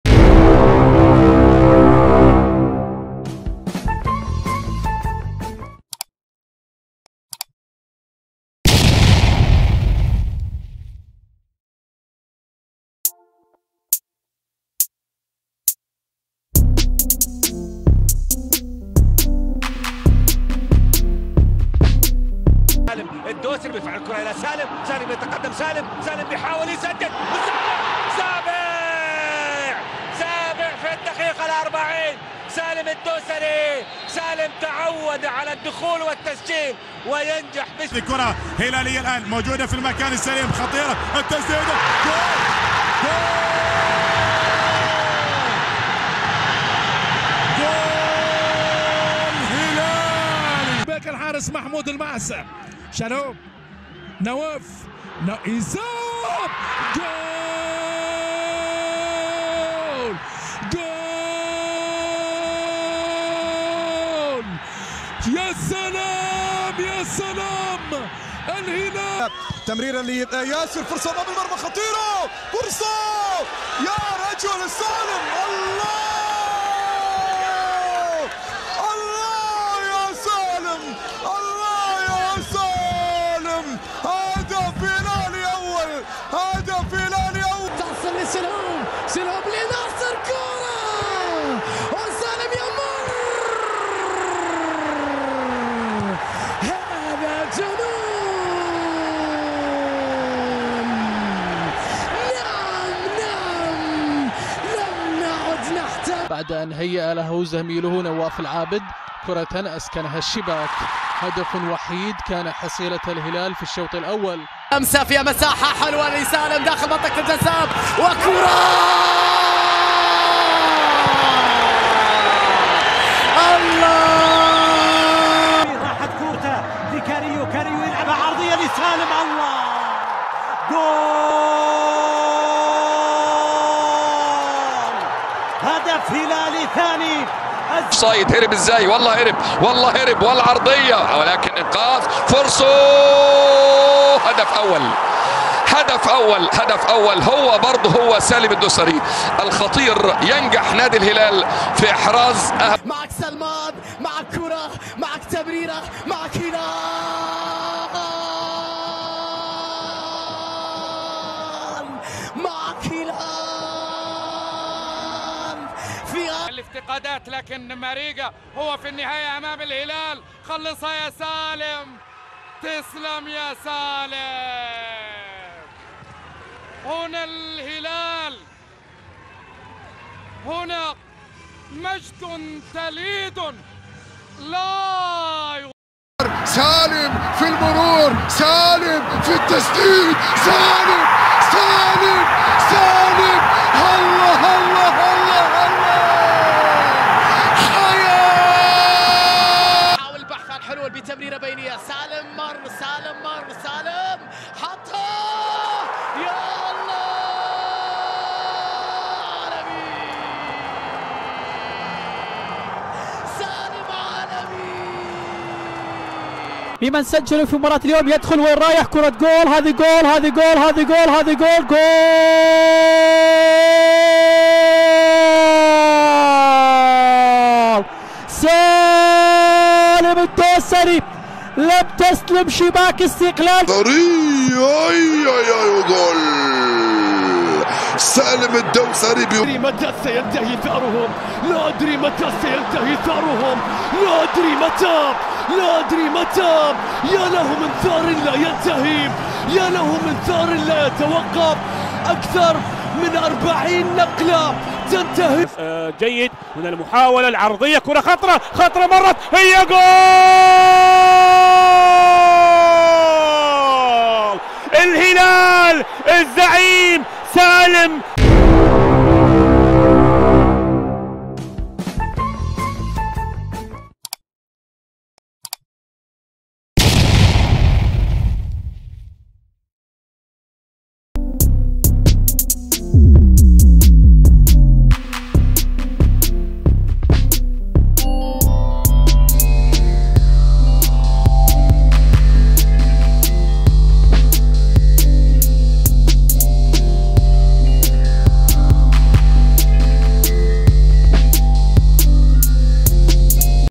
أهداف سالم الدوسري في دوري أبطال آسيا
أهداف لاعب الهلال و منتخب السعودية , زعيم آسيا , سجل في الإتحاد والنصر و أوراوا الياباني و الشارقة والعين وبني ياس  الإماراتي